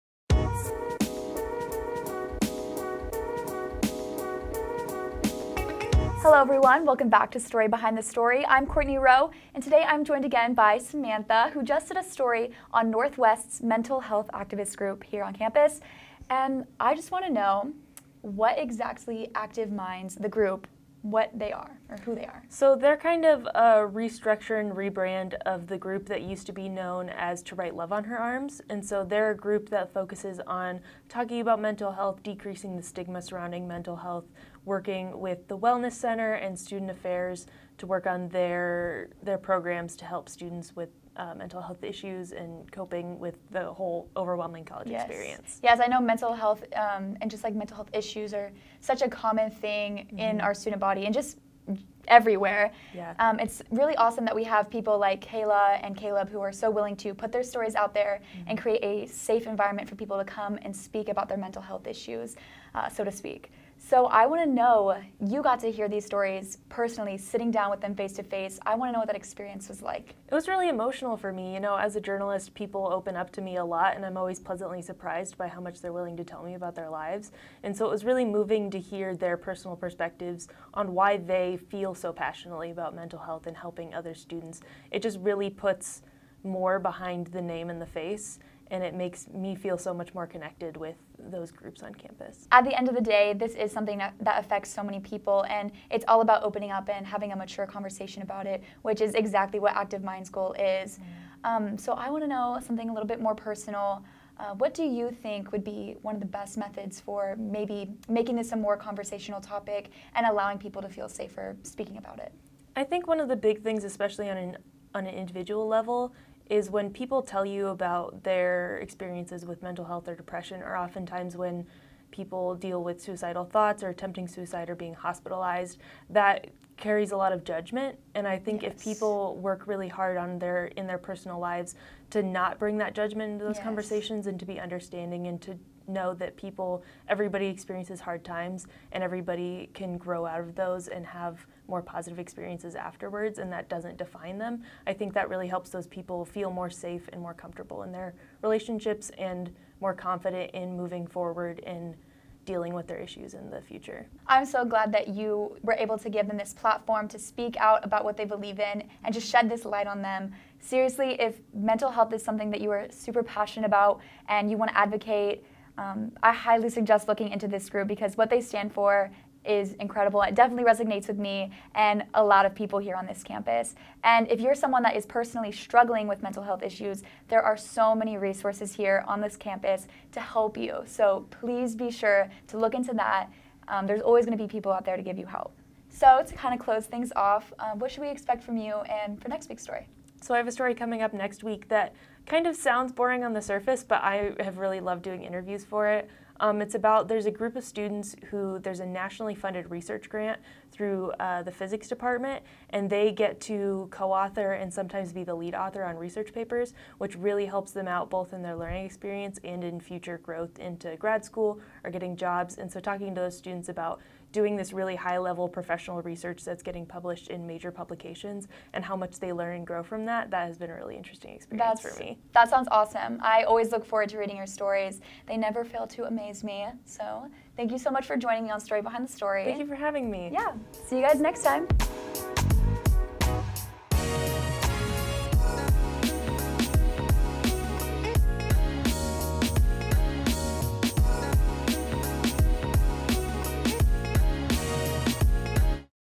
Story Behind the Story | Week 4: Samantha Collison
Sammie's story highlights a new mental health activist group on campus.

KNWT provides student-produced original programming to the campus and community surrounding Northwest Missouri State University.